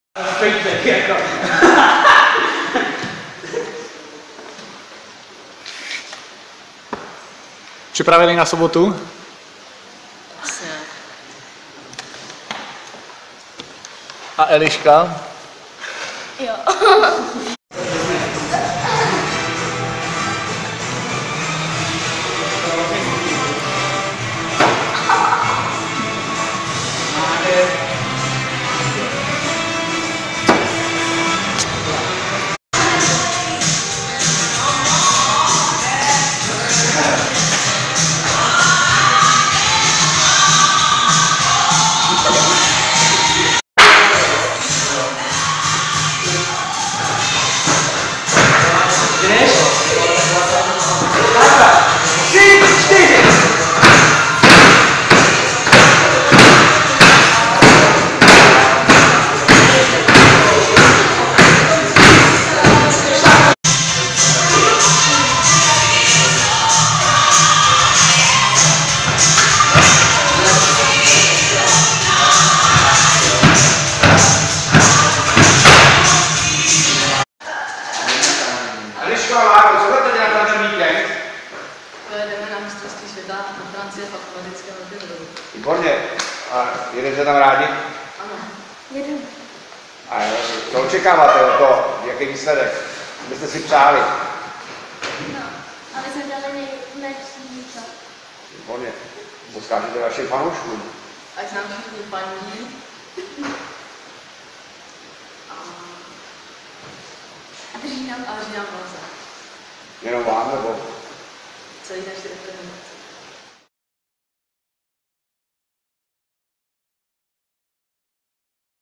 Marek a Eliška - Pozvánka na Mistrovství světa 2015
Pozvánka na Mistrovství světa v akrobatickém rokenrolu od Marka a Elišky.